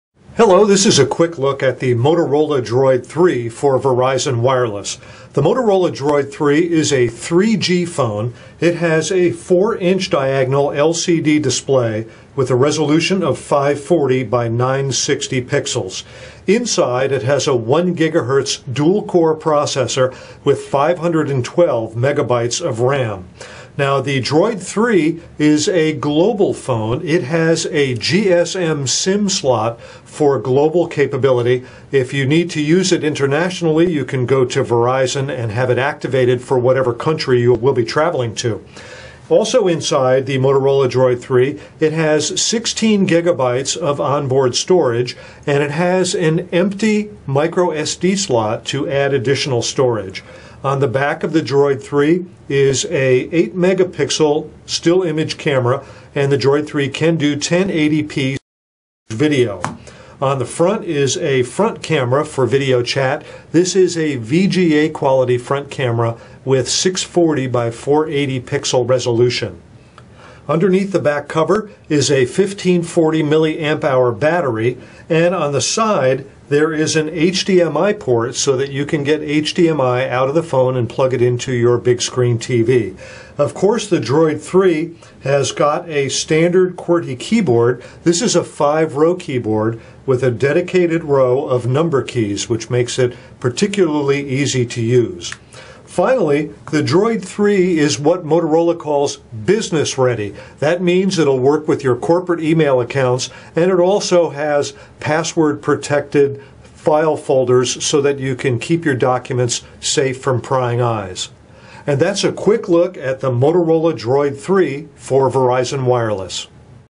Motorola DROID 3 Quick Look
This is a quick look at the Motorola DROID 3 for Verizon Wireless.